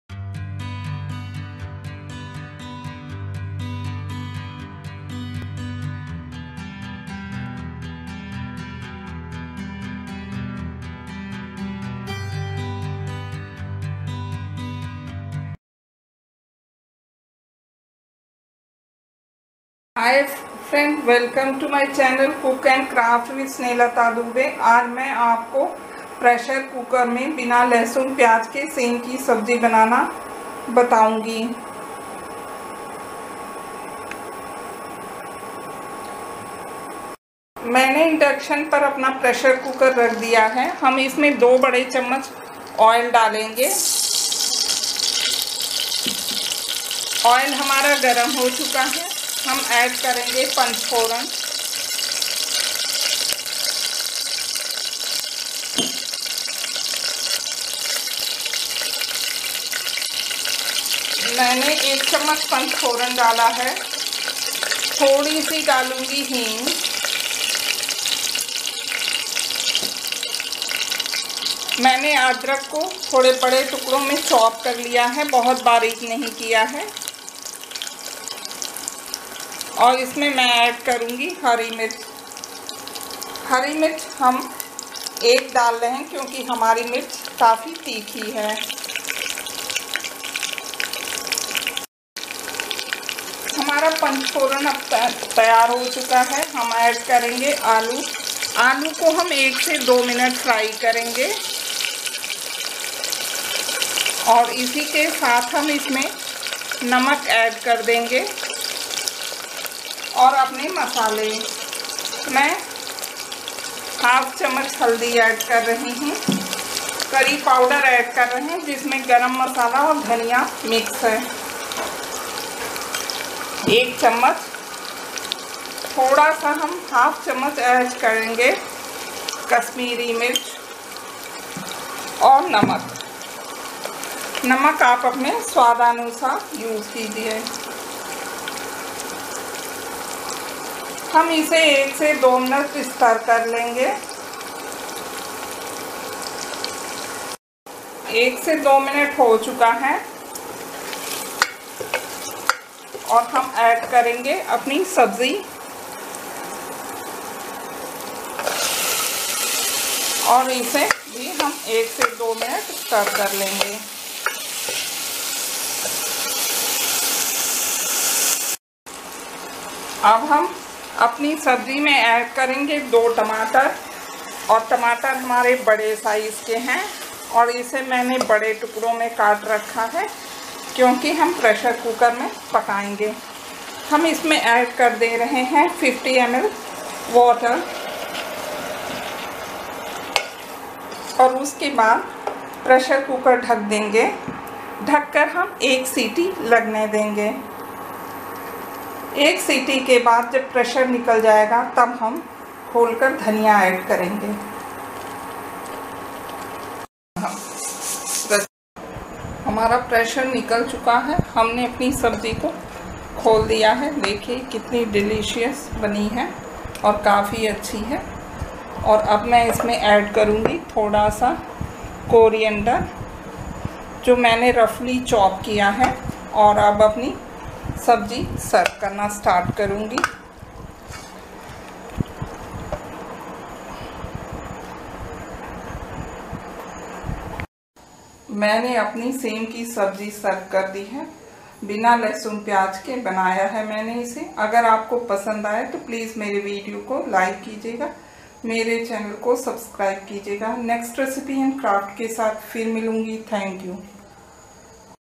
Broad Beans Curry /सेम की सब्ज़ी
Aalu - Sem ki phalli ki sabzi without onion and garlic
                in pressure cooker | Board  beans curry
Ingredients :
1 . Sem ki phalli | Broad bean-200g       2 . Potato -2pcs
3 .Chilli -1 pcs                                        4 .Panch phoran -1 Tbsp
5 .Curry powder -1 Tbsp                        6 .Turmeric powder -1/2 Tbsp
7.Chilli  powder -1/2 Tbsp                      8.Ginger -1 inch
9. Hing – 2 pinch                                   10.Tomato -2 pcs
11.Salt as per taste                               12.Coriander  leaves.